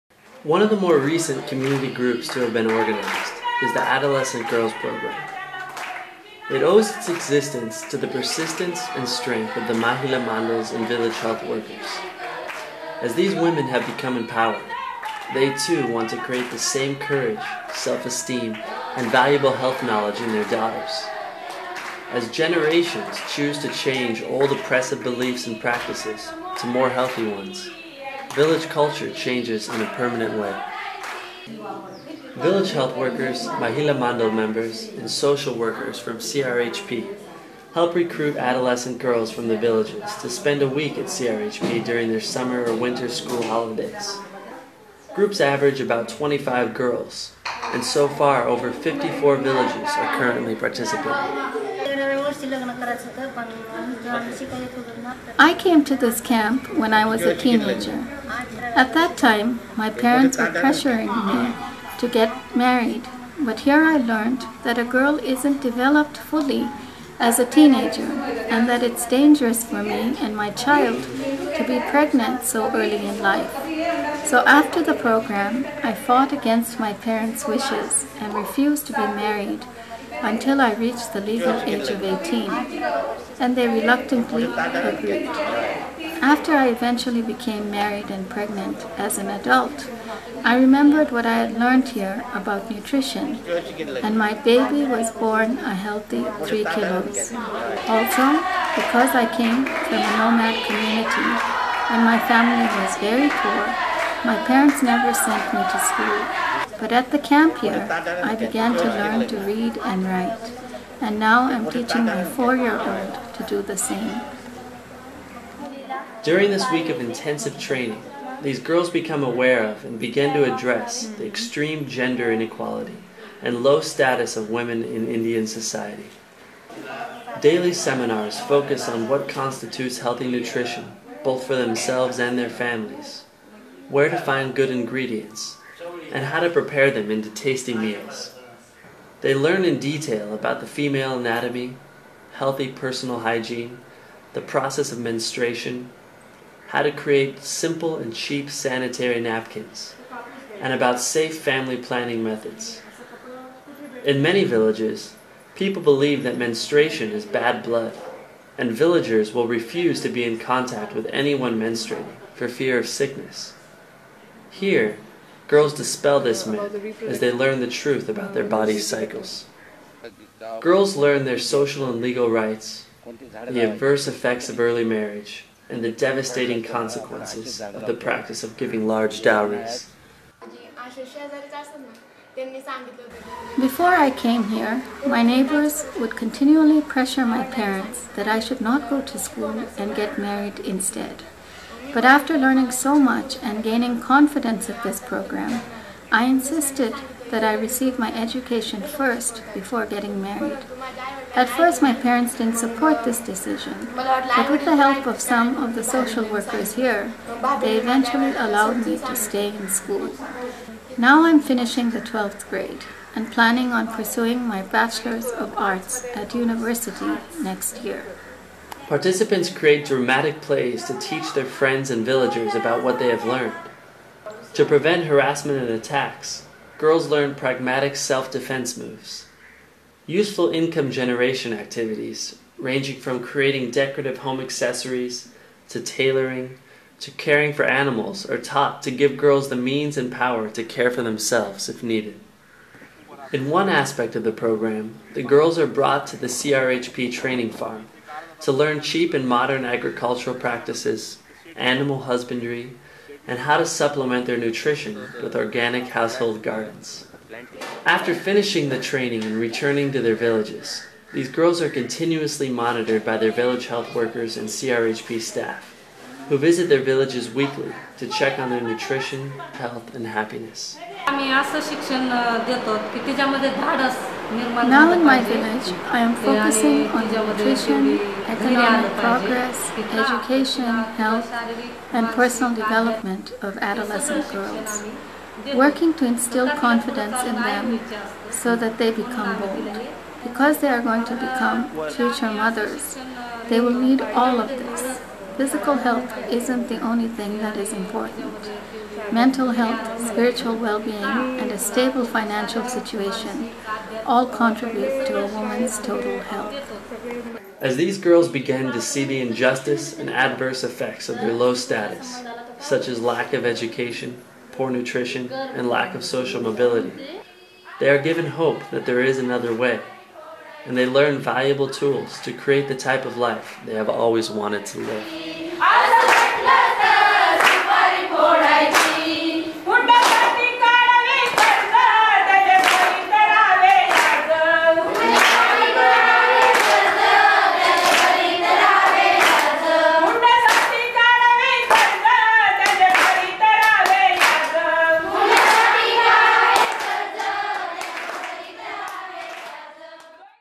Jamkhed CRHP Adolescent Girls Program
The Adolescent Girls Program (AGP) is one of the latest of the community groups to have been organized. This program owes its existence to the presence and strength of the Mahila Mandals and VHWs. These women have been empowered to realize the need to educate, build up the self-esteem and confidence in their daughters. In effect the entire community benefits as health and development are promoted by a new generation of knowledgeable, competent, and socially-minded women. The AGP has helped organize adolescent girls groups in 54 villages with an average of 25 girls/village participating.

The need for an adolescent girls program is to address the extreme gender inequity and the low status of women in Indian society, particularly in the rural areas. Girl children are given far less opportunities than boys and are considered a burden on the family due to the eventual need for dowry and their marriage out of the family. They are consequently disadvantaged in such areas as education, nutrition, health care, employment and social mobility. The practice of sex-selective abortions and female infanticide is another manifestation of women's poor social standing and has resulted in highly skewed gender ratios throughout India, particularly in the North. Early marriage, sometimes prior to puberty, often results in early sexual initiation and teenage pregnancy thereby compromising education and livelihood choices. Such are the factors that are being addressed and even reversed through the participation of girls in the AGP.

In the project areas of CRHP female adolescence is culturally understood as the time between menarche and marriage (regardless of age) known as  Kishorvain muli. This criterion is used by CRHP to identify participants for the AGP. Related activities take place in either of two locations -- individual villages and the CRHP compound.

In the villages VHWs and women's group members organize groups of girls, which meet weekly or biweekly. During these meetings the girls participate in health education classes as well as being given the rare opportunity to socialize. Organized groups are given some books for a library and provided with high-protein and nutritious food for their meal as a nutrition demonstration. The health of these girls is consistently monitored by the respective VHWs and mobile health team, addressing problems as needed.

Groups from several villages occasionally come to the CRHP compound where they stay overnight. The groups are accompanied by the VHW from their villages.